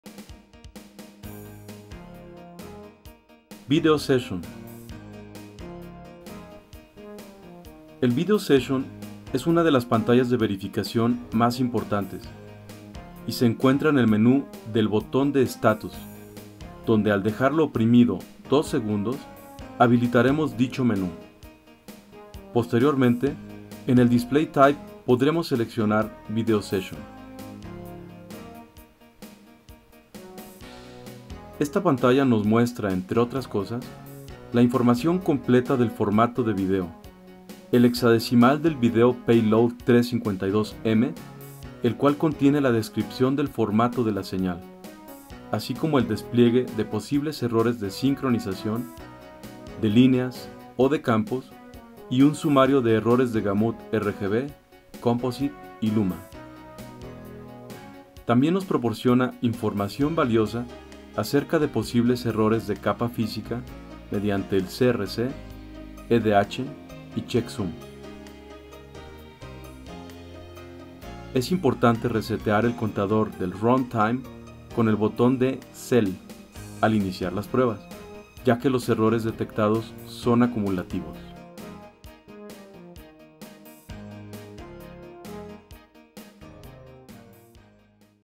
Medición en el Video Session
Breve explicación de configuración y operación en el Monitor Forma de Onda Tektronix.